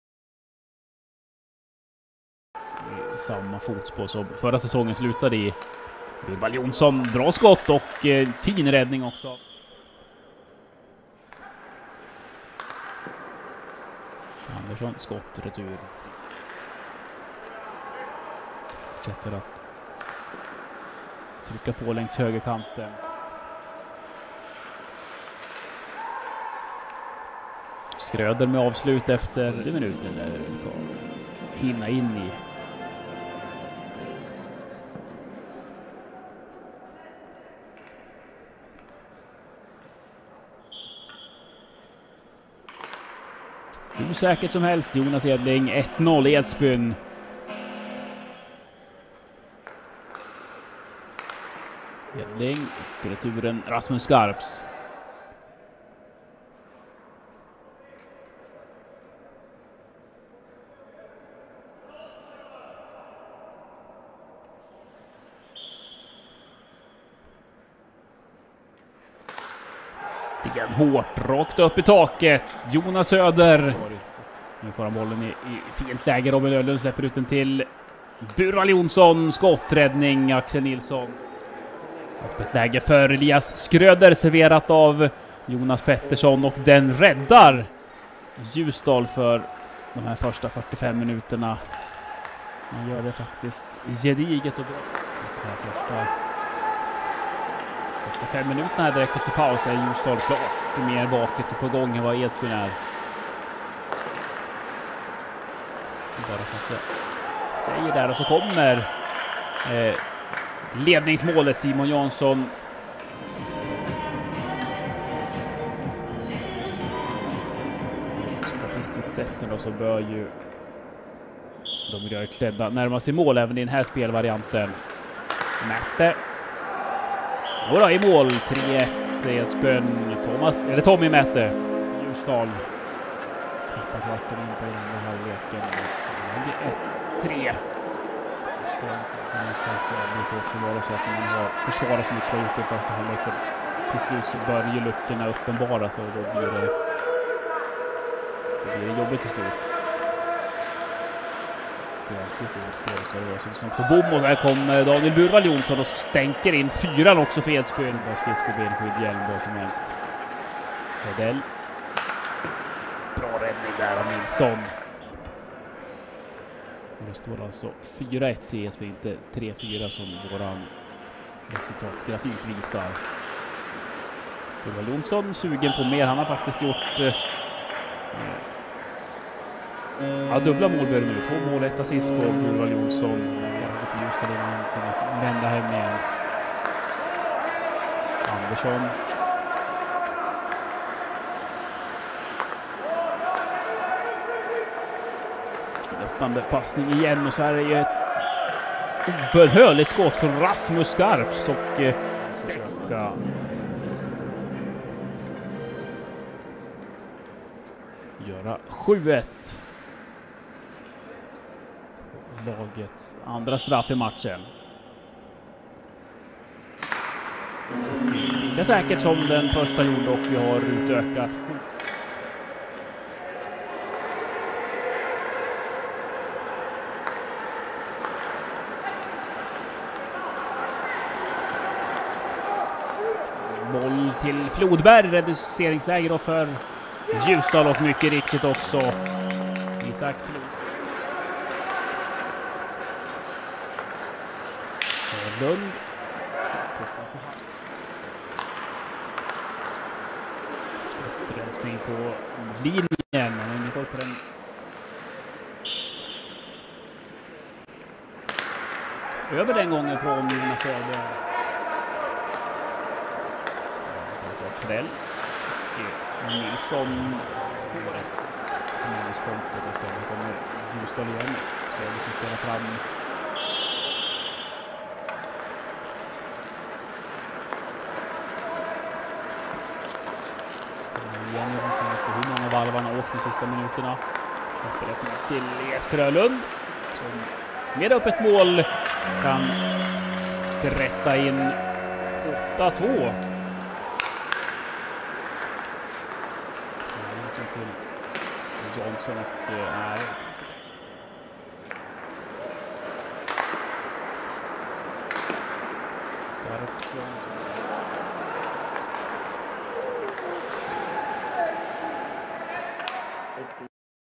12/09/20/Match om 3-4 plats/«Ljusdals BK»-«Edsbyns IF»/Champions Cup-2020/
MATCHFAKTA
Ljusdal–Edsbyn 2:8 (1:1)

Mal: 0–1 (27, straff) Jonas Edling, 1–1 (39, straff) Jonas Soder, 1-2 (50) Jesper Ohrlund (Simon Jansson), 1-3 (57, horna) Tommi Maatta (Daniel Burvall Jonsson), 1–4 (58) Daniel Burvall Jonsson, 1–5 (60, horna) Daniel Burvall Jonsson (Hans Andersson), 1–6 (63) Rasmus Skarps (Hans Andersson), 1-7 (69, straff) Jonas Edling, 2–7 (70) Isak Flodberg (Elias Swartswe), 2-8 (87) Jesper Ohrlund (Simon Jansson).

Hornor: 13–16 (2–9)
Domare: Pavel Silinski